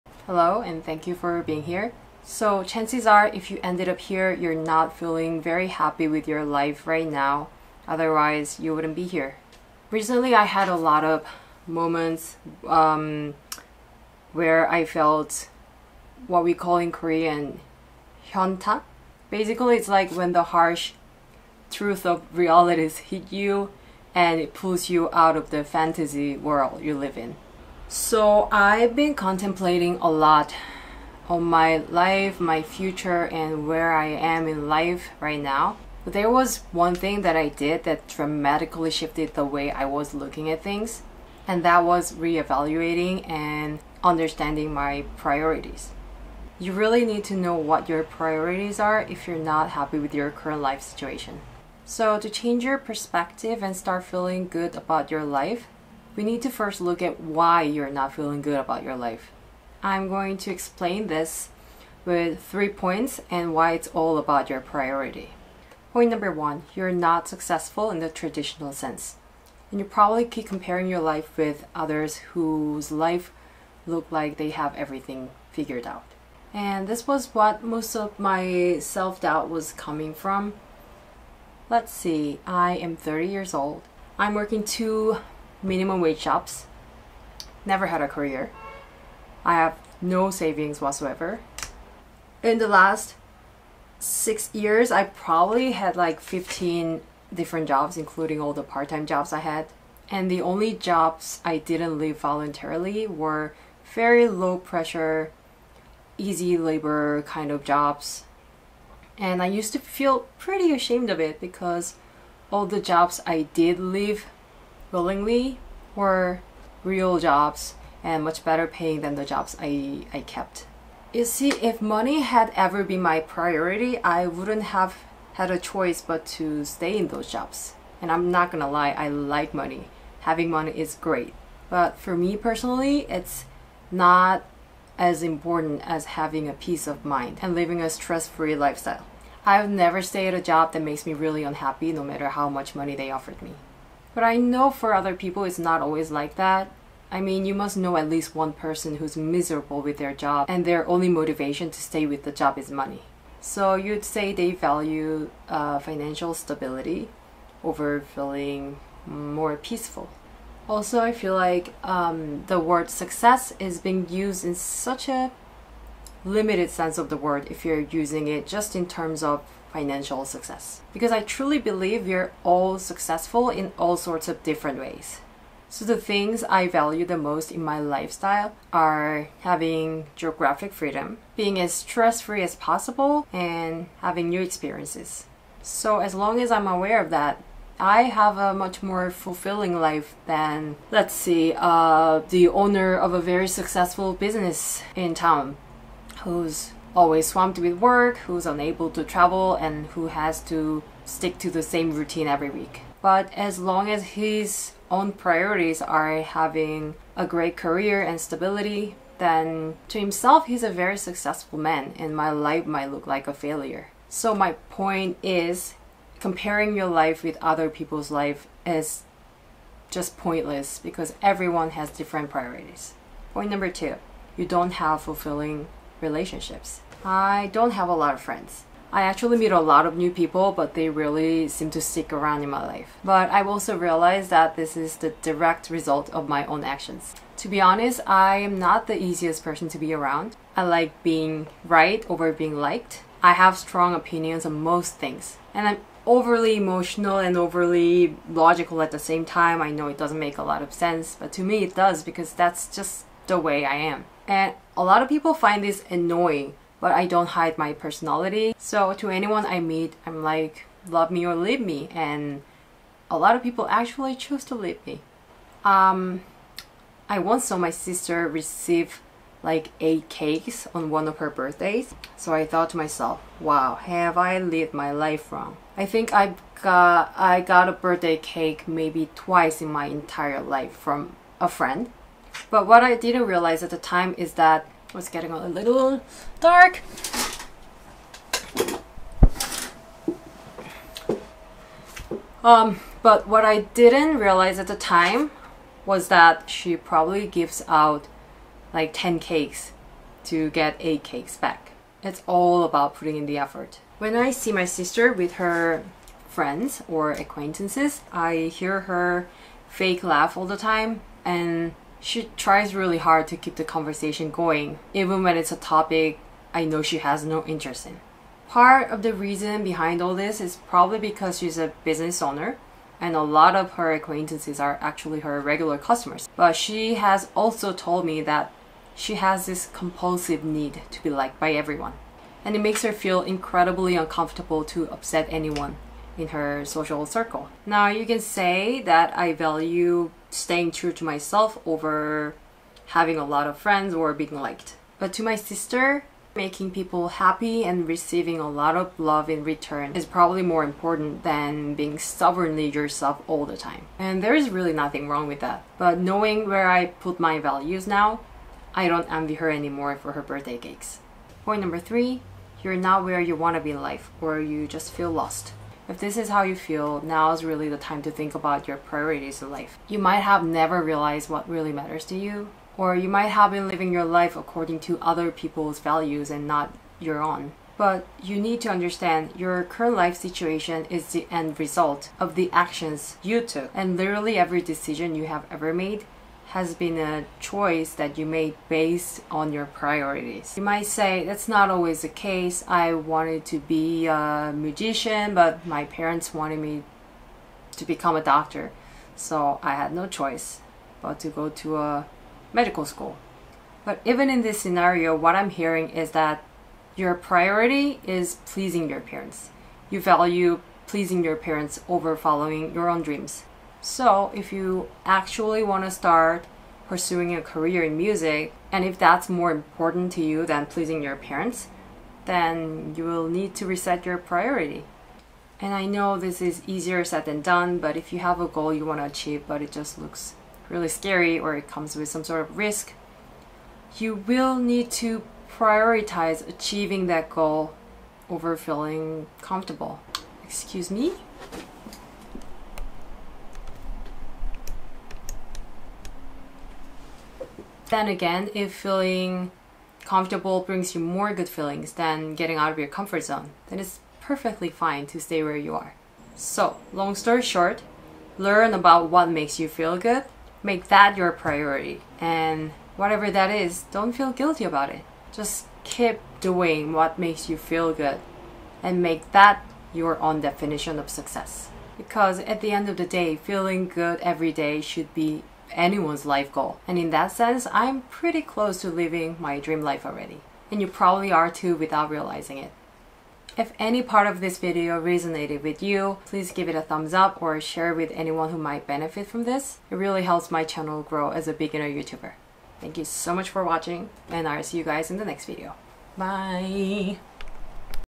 how to feel good about your life with no achievements
Hello friends and welcome!
In this video I share my approach to looking at things from a different, healthier perspective to feel better about your life and ultimately, yourself.

0:00 Know your priorities
1:06 You don't have a "successful" career
3:42 You don't have fulfilling relationships
6:02 You don't know what's important to you